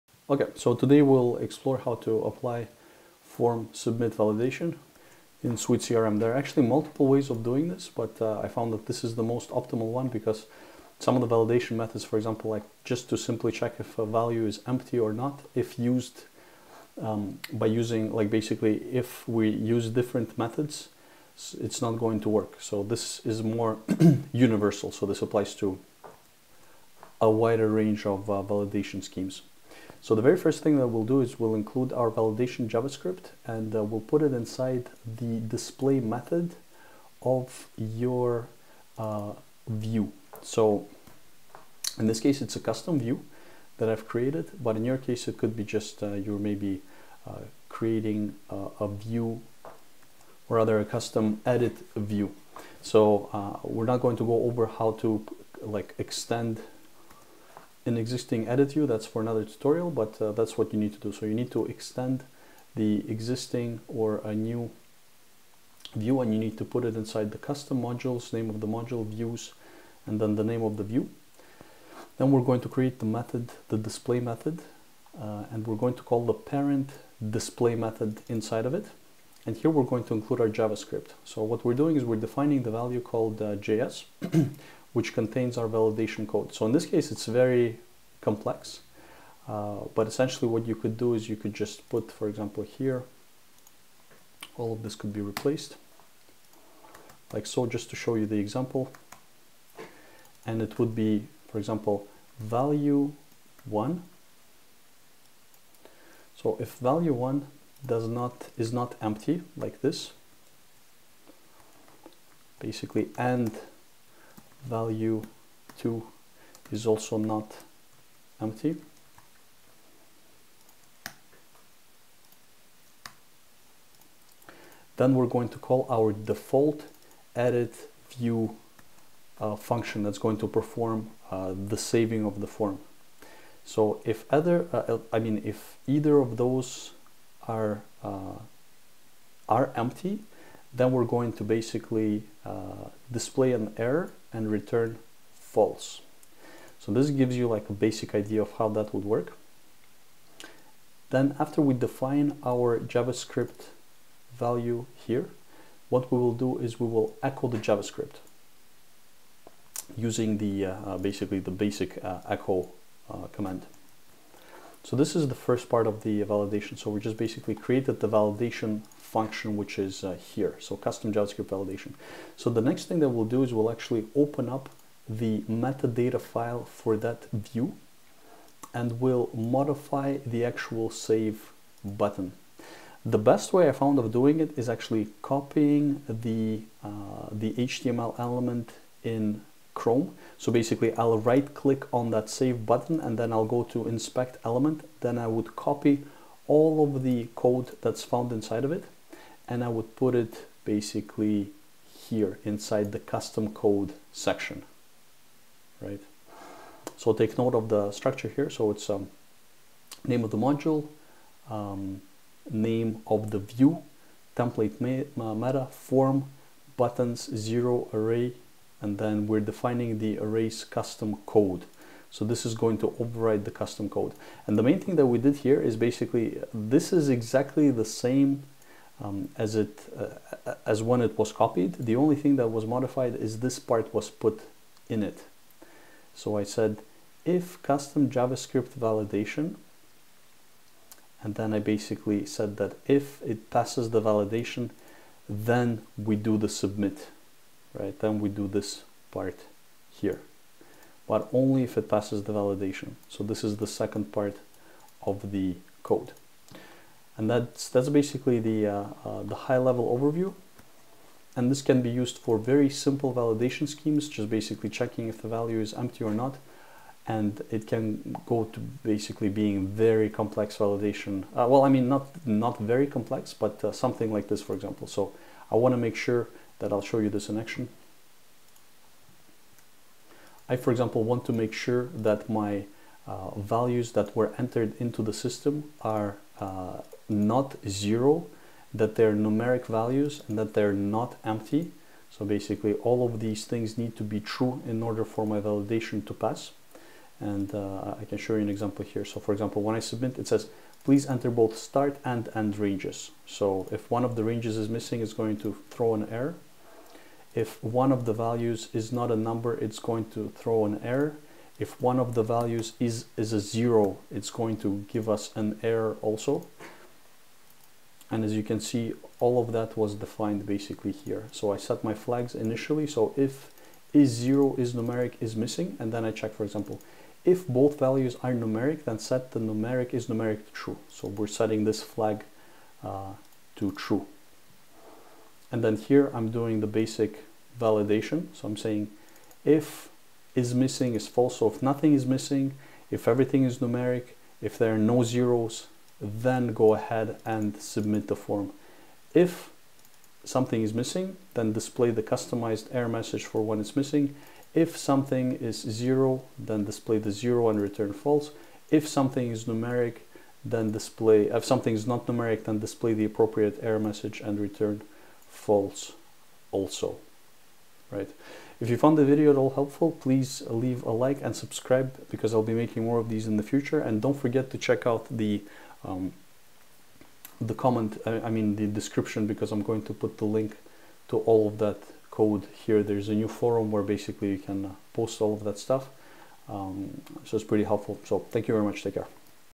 Form validation in suitecrm
Basic flow is to modify the display() method and insert your validation function. Then modify the metadata file and insert custom javascript into the save button so it calls the validation function.